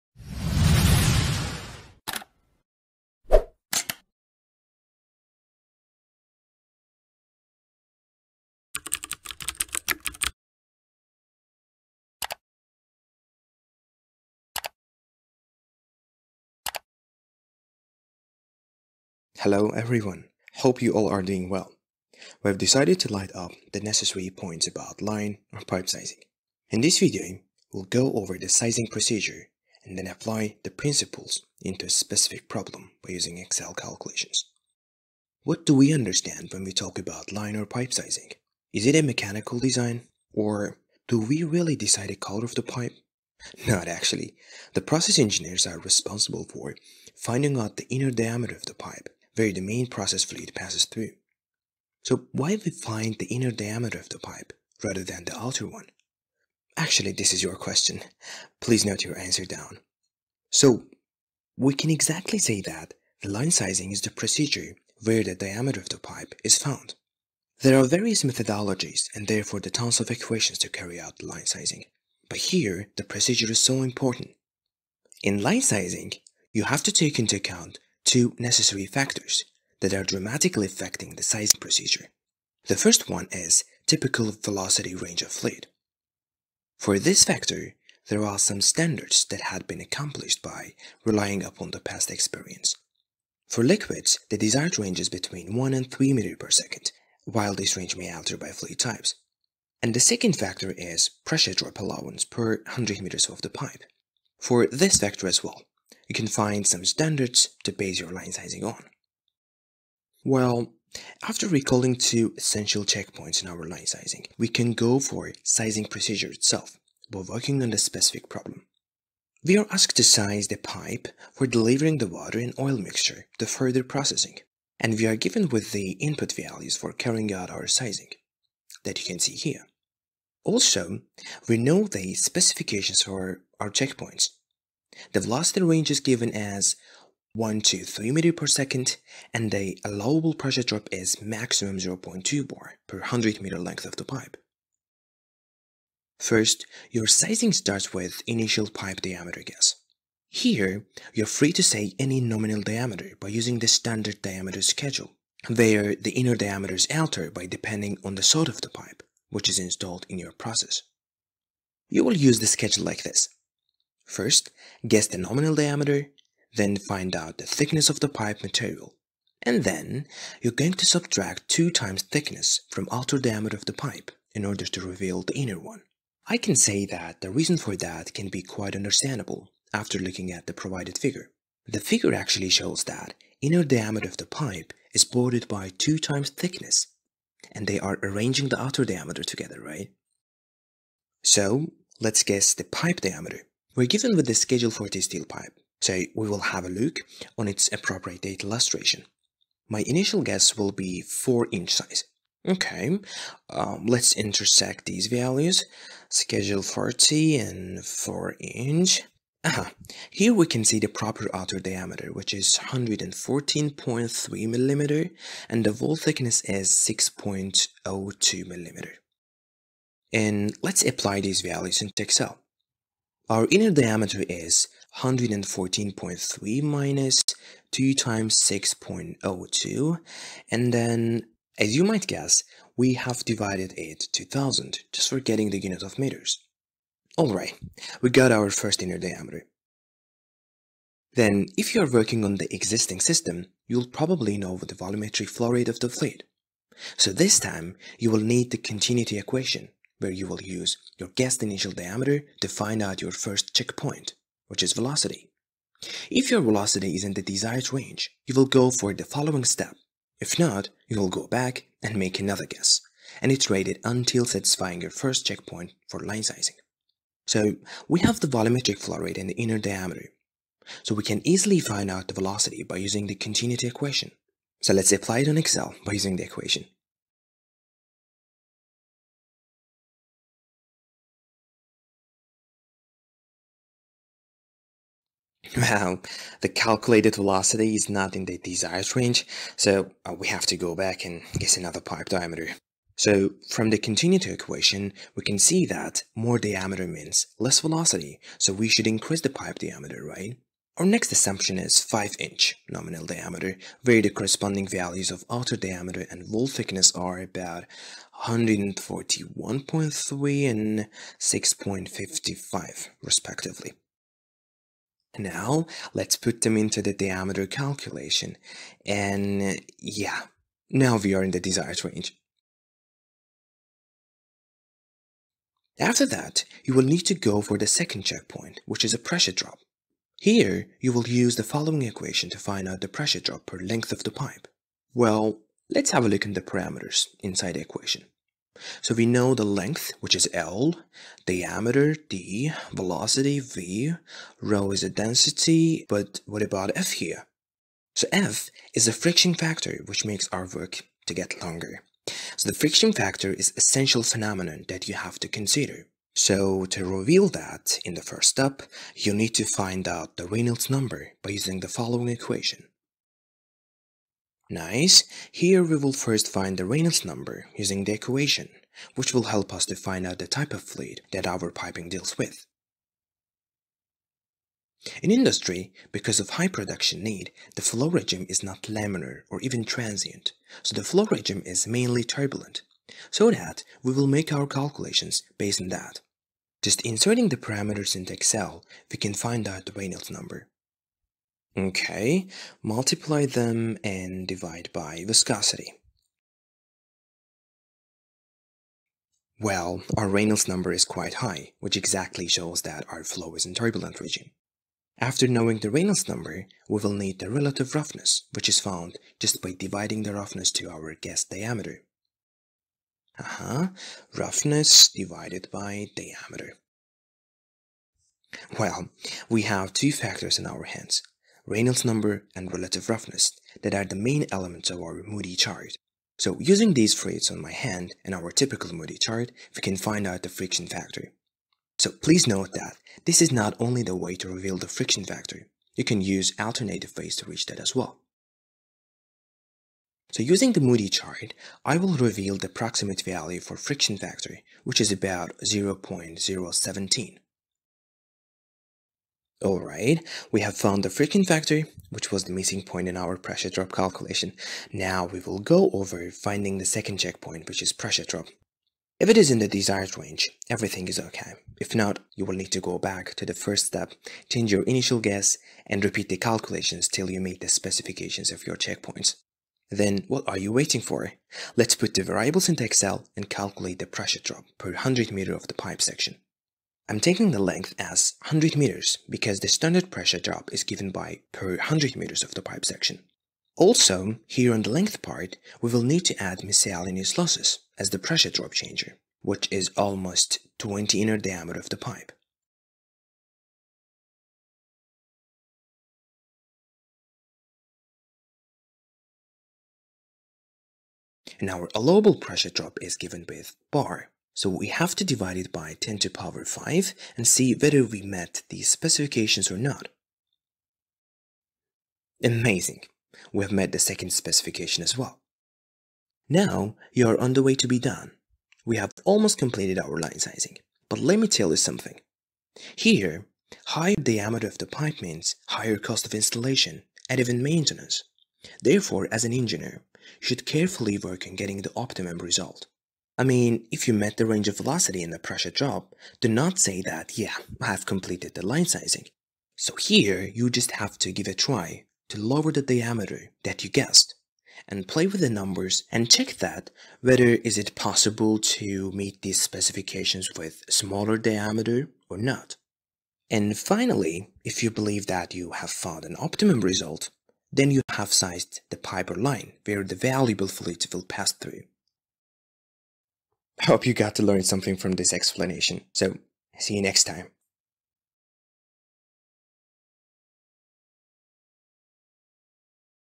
Process Engineering| Line/Pipe Sizing Informative Session
Fluid flow and dynamics should be regarded as the most significant element of process engineering which aims to analyze its type, physical properties, external constraints and etc. The practical side of fluid flow fundamentals is ready to be welcomed by our dear followers! Here in this video, you will go over:
 - The significance of pipe sizing and its practical sides;
 - Precise line sizing principle and
 - Two mandatory factors that affect the sizing of line or pipe
Follow the new content on Velociss Learning Platform and our Udemy Channel to get more about Fluid behavior through a straight pipe: